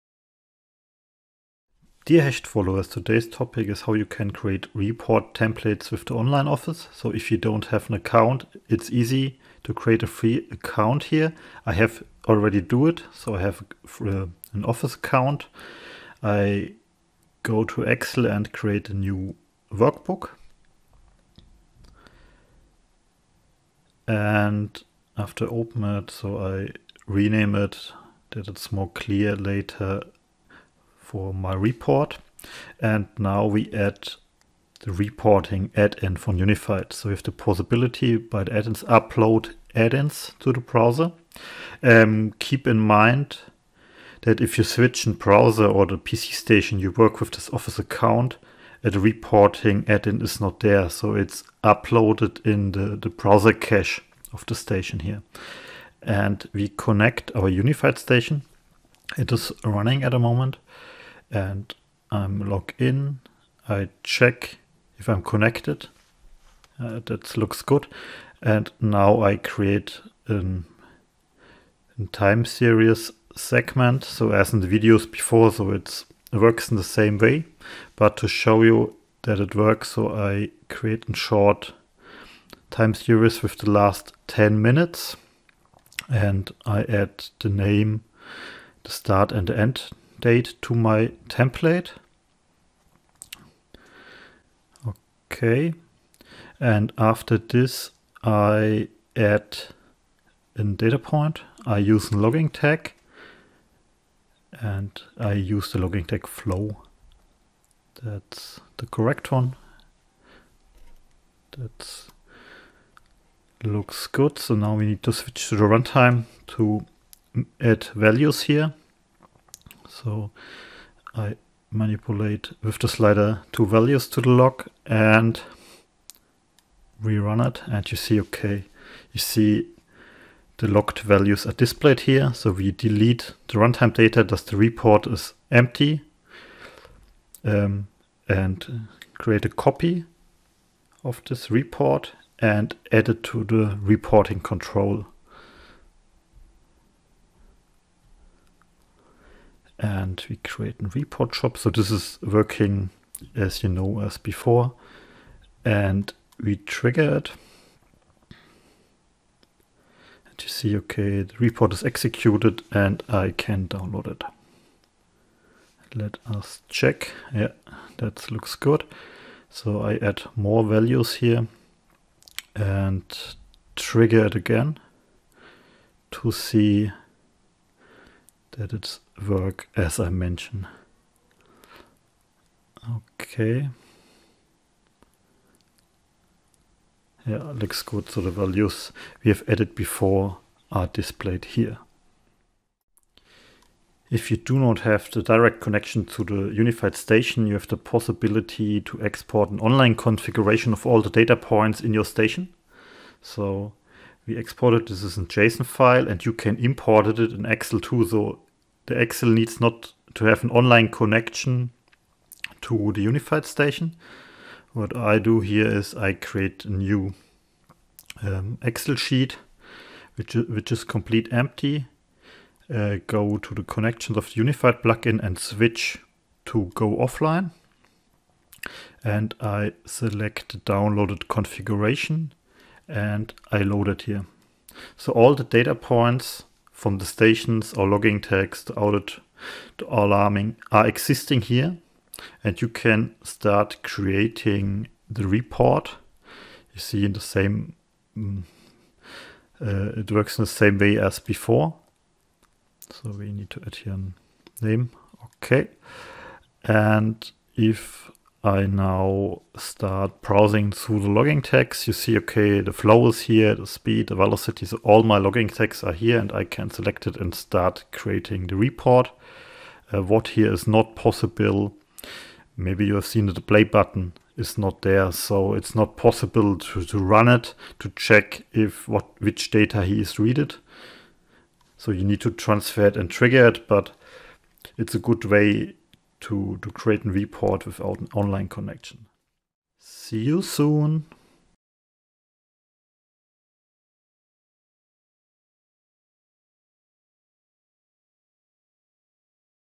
WinCC Unified: Create reports with the Online Excel and create a offline report
WinCC Unified: Create reports with the Online Excel

In this Video I show you how you can use the free online Excel to created a report.
Furthermore how it is possible to use the offline configuration to set up a report without a connection to the Unified project

00:00 Intro
00:37 add the Unified Plug-Inn
01:10 create a report
02:20 trigger a report
03:18 download a offline configuration
03:41 create a template with offline data